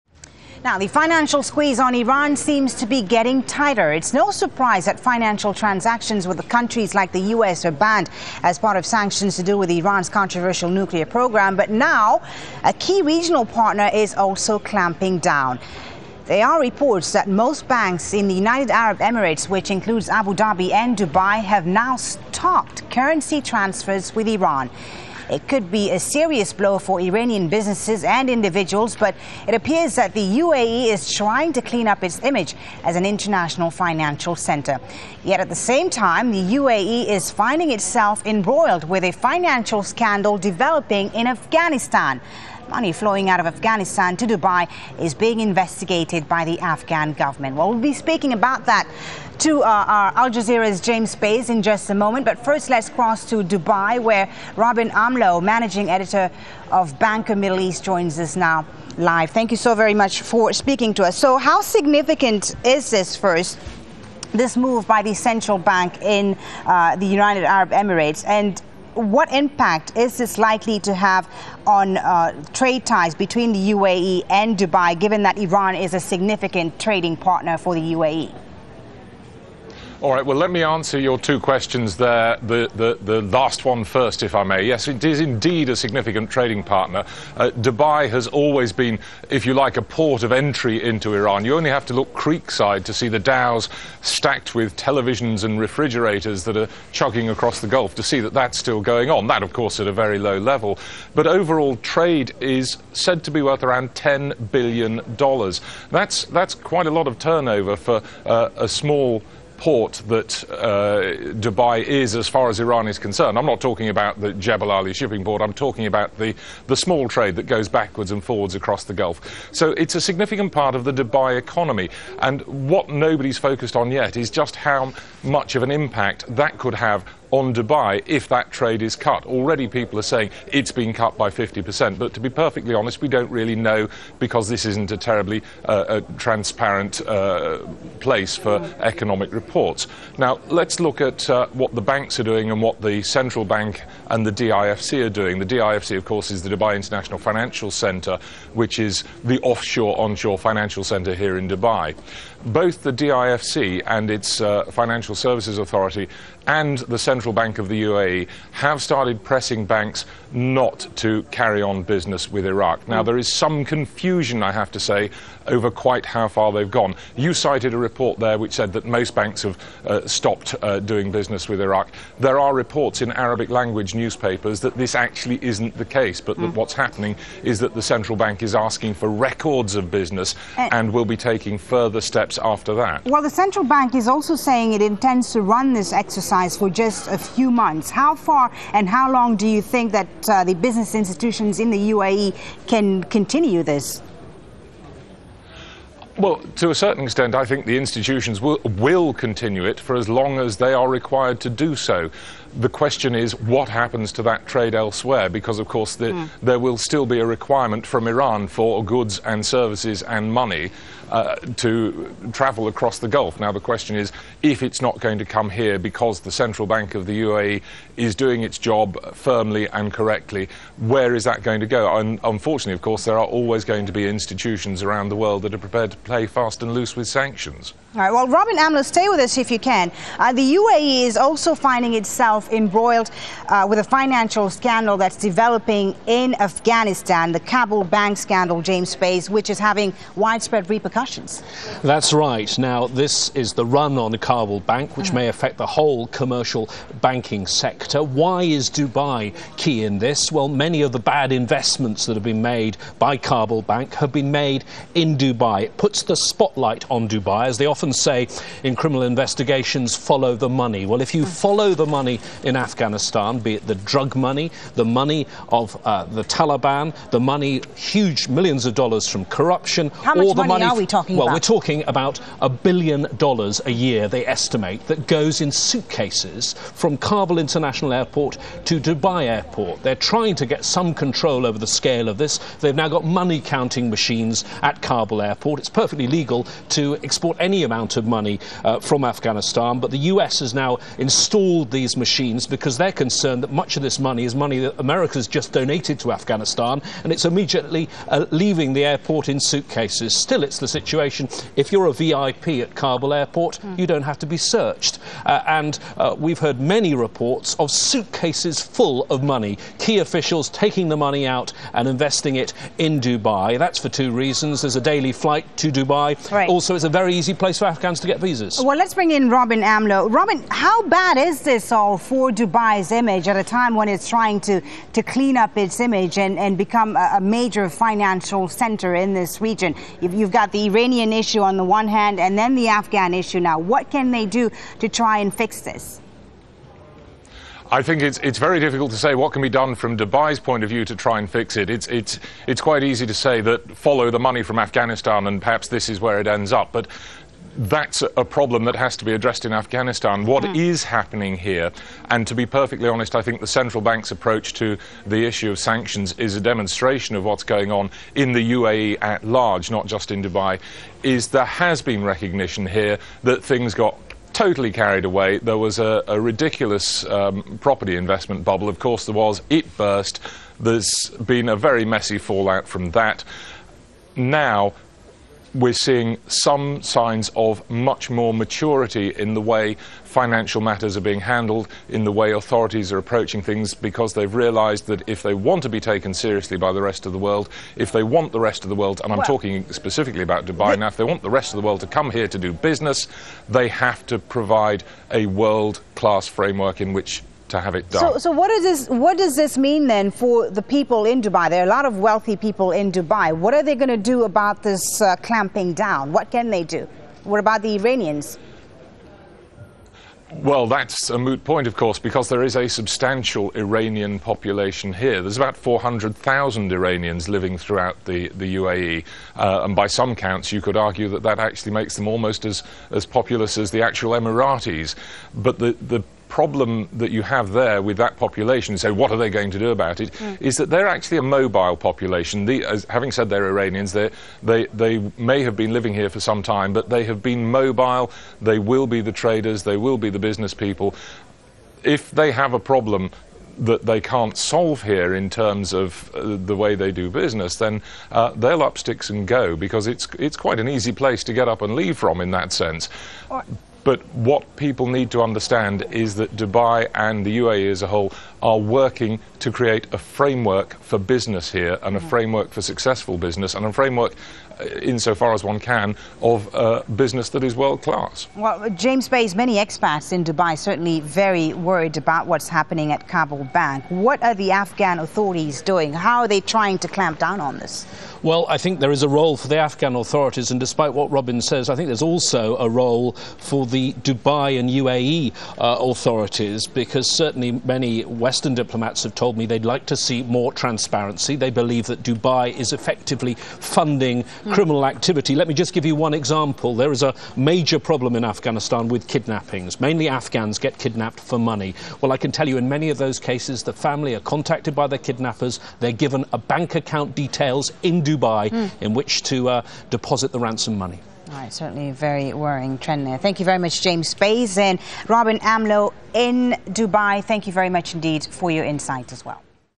UAE acts on money tranfers to Iran
It is no surprise that financial transactions with countries like the United States are banned as part of sanctions to do with Iran's nuclear programme, but now a key regional partner is also clamping down.

The central bank of the United Arab Emirates (UAE) has advised banks in the federation, including those in Abu Dhabi and Dubai, to stop currency transfers with Iran.

This could be a serious blow for Iranian businesses and individuals, but it appears that the UAE is trying to clean up its image as a international financial centre.

At the same time, the UAE is finding itself embroiled with a financial scandal developing in Afghanistan.

Money flowing out of Afghanistan to Dubai is being investigated by the Afghan government.

Al Jazeera spoke to Robin Amlot, managing editor of Banker Middle East, in Dubai and James Bays, Al Jazeera's Afghanistan correspondent, about the issues.